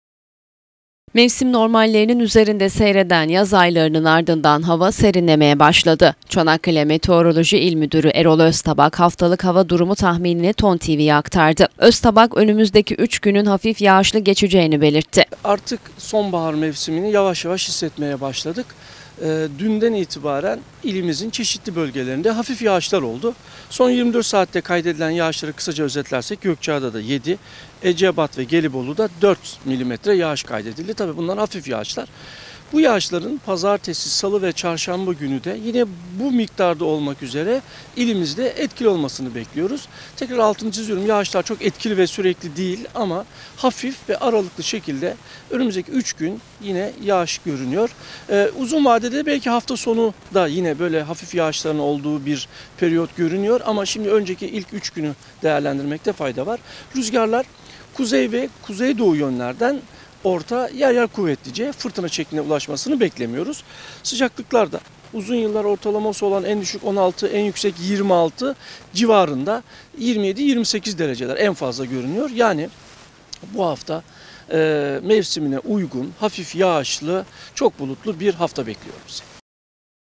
ÇANAKKALE'YE YAĞIŞ GELİYOR
Çanakkale Meteoroloji İl Müdürü Erol Öztabak, önümüzdeki üç gün kentte yağış beklendiğini açıkladı.